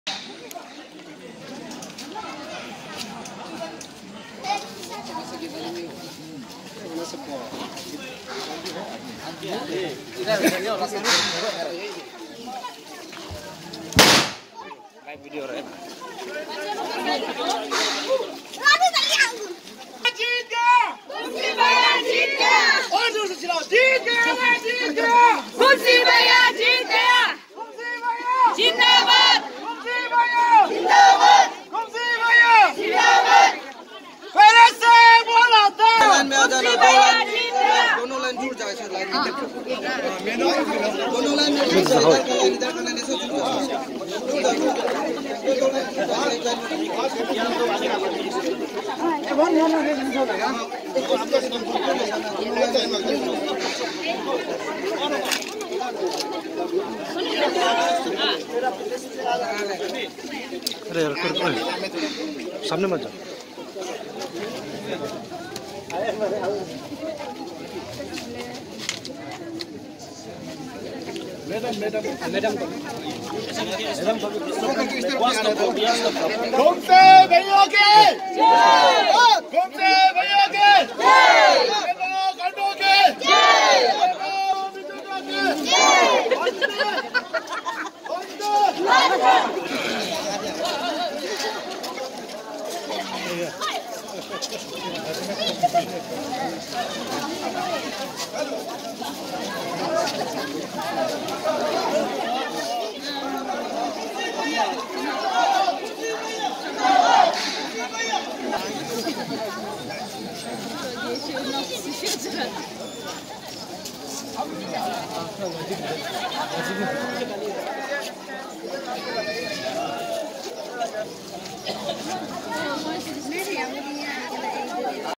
Victory Parade - Kumsi Sidisow HMLA 6 th Thrizino -Buragaon AC
People of Arunachal Pradesh welcomes their leader with open hearts and high hope at Bhalukpong